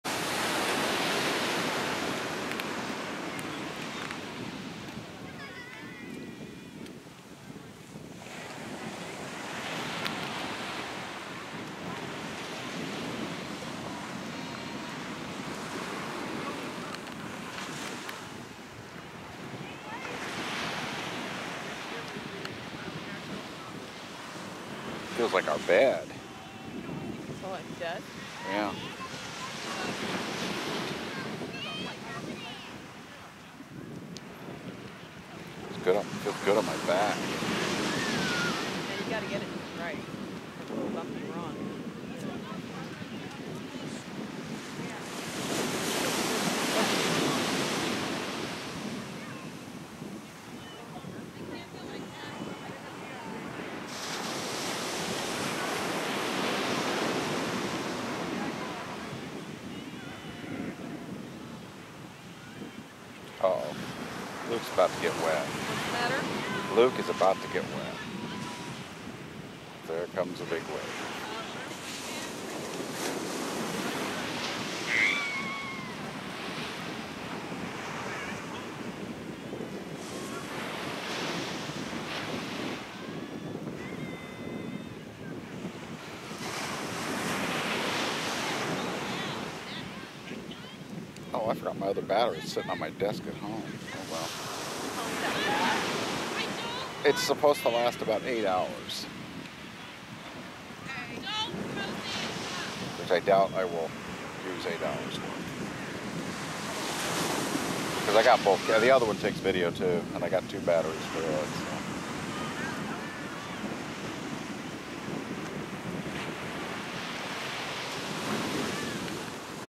A day at the beach (the knee to the left of the screen is my knee). They weren't going to go in the water. That was what they said. Within 5 minutes they were soaking wet, in the water!